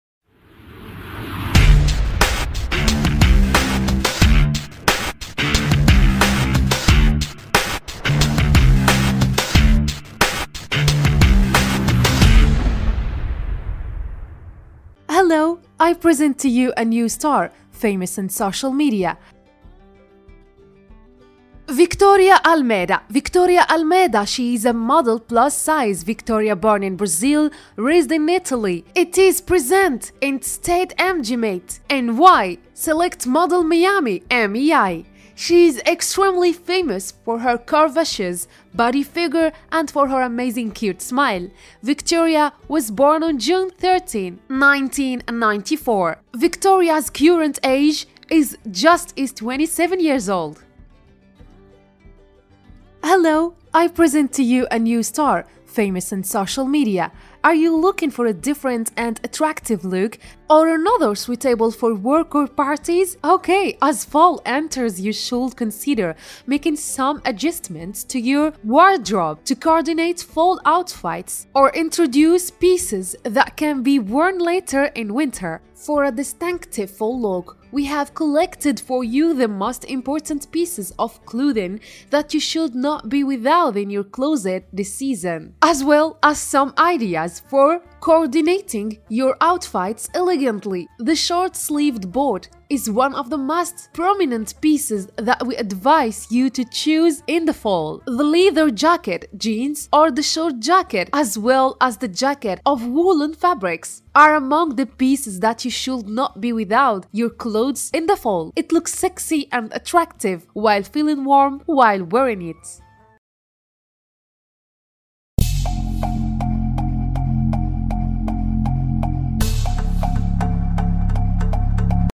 Vitoria Almeida .. Wiki Biography, body measurements, age, relationships
vitoriaalmeiddaa Instagram Model Fashion nova & Biography and Lifestyle

 Vitoria Almeida sports two swimsuit looks from the Fashion Nova Curve collection.

ـــــــــــــــــــــــــــــــــــــــــــــــــــــــــــــــــــــــــــــــــــــــــ
LEGAL INFORMATION ABOUT THE CONTENT:
ــــــــــــــــــــــــــــــــــــــــــــــــــــــــــــــــــــــــــــــــــــــــــ